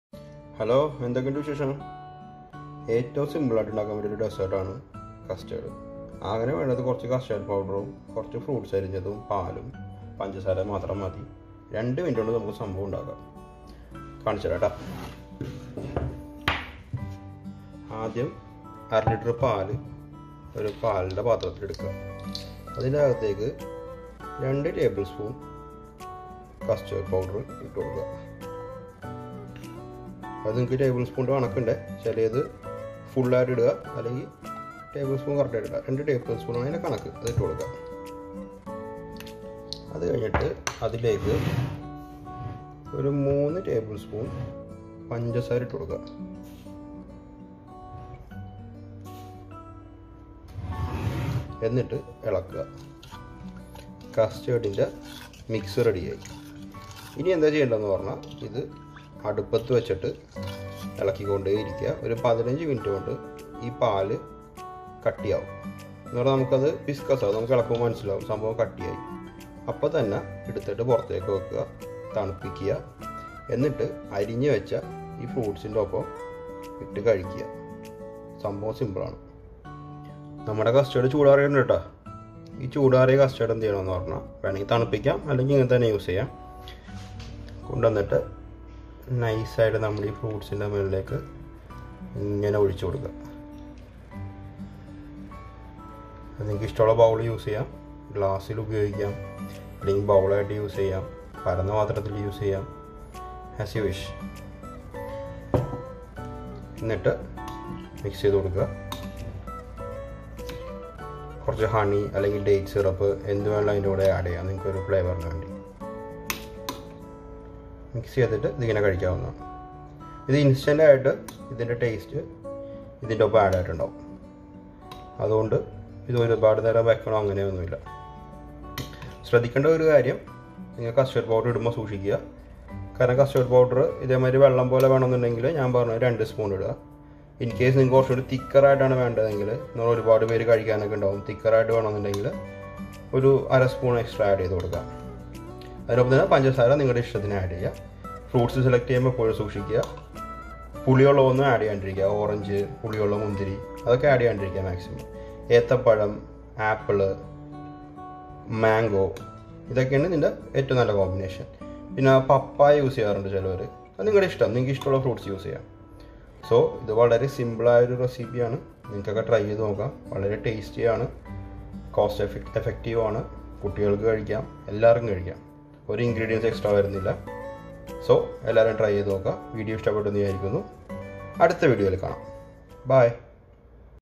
Custard Fruit Salad- Malayalam
Easy preparation of simple and healthy custard dessert.